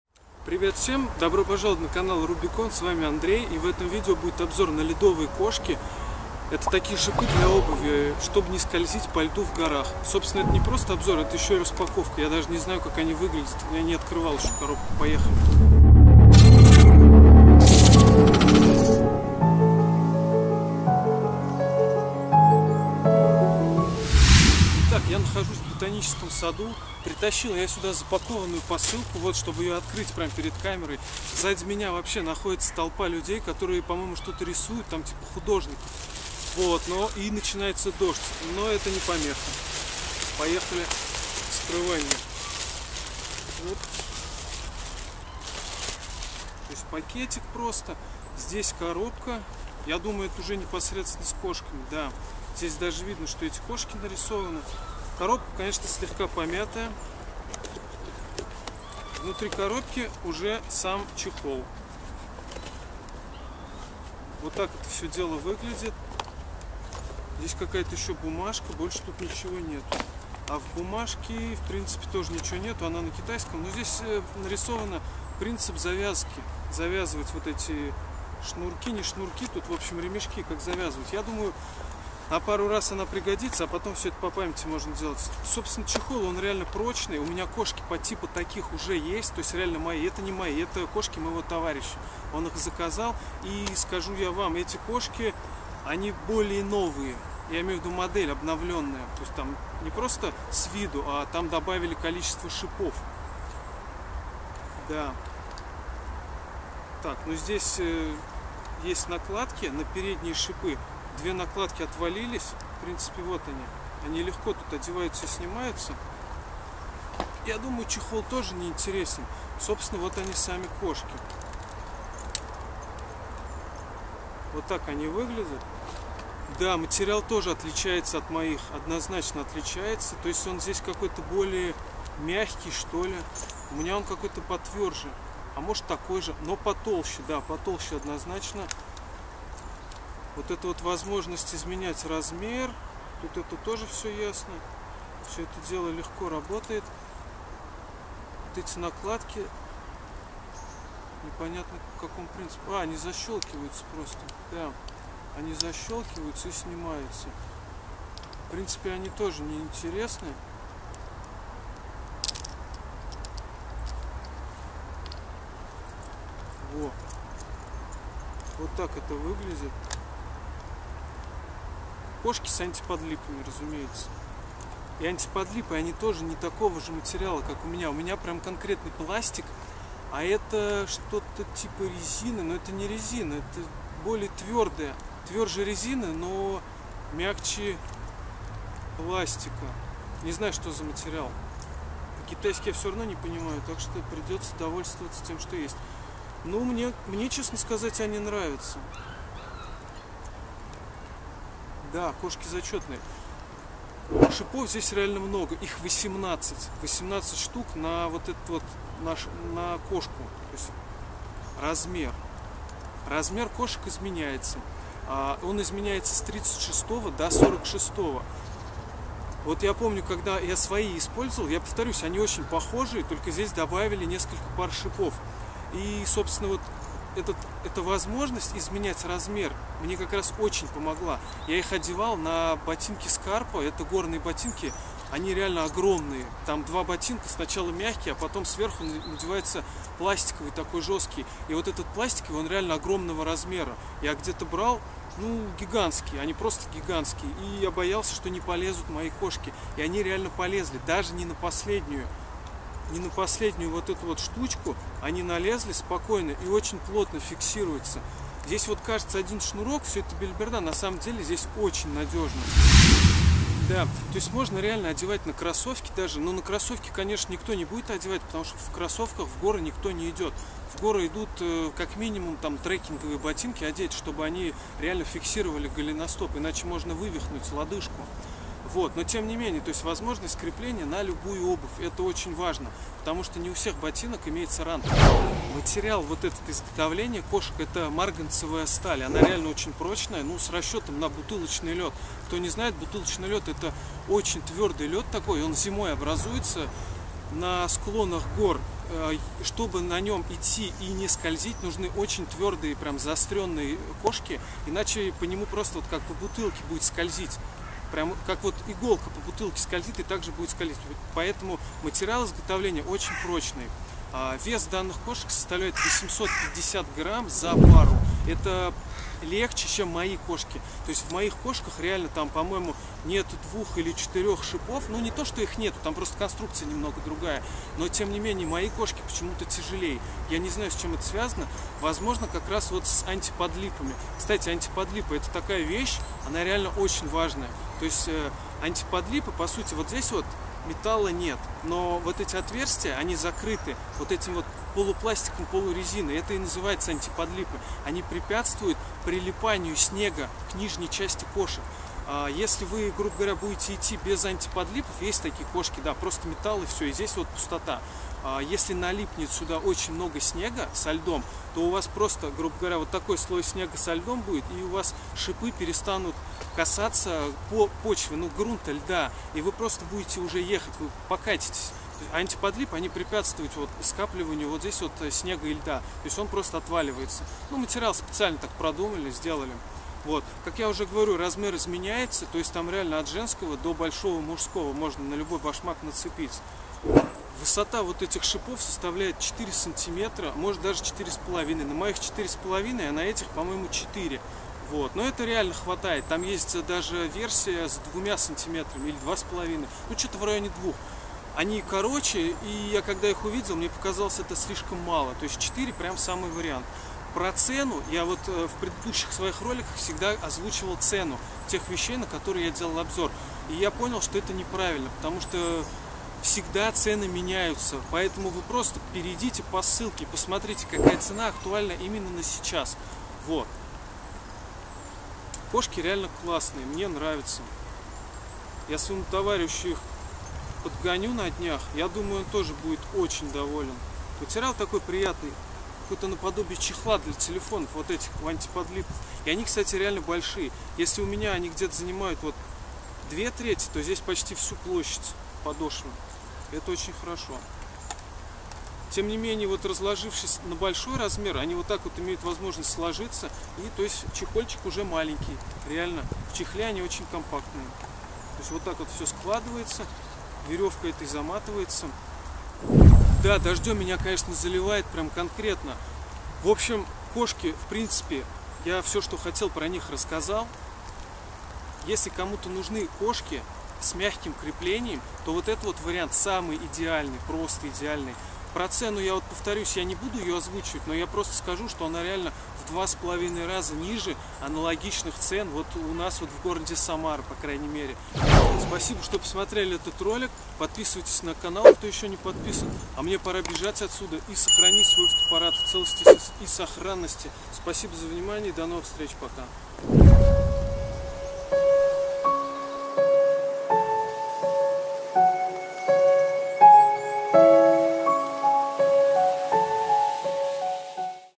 Альпинистские Кошки с Мягким Креплением. Распаковка, Обзор.
Обзор Альпинистских кошек с мягким креплением с Алиэкспресс. Распаковка посылки из Китая.

Кошки, кошки альпинистские — металлические приспособления для увеличения противоскольжения при передвижении по снежно-ледовому рельефу. В современном виде представляют собой металлическую пластину из высокотехнологичных металлических сплавов с 10/12 клиновидными зубьями, имеющую определённую систему крепления к обуви. Википедия.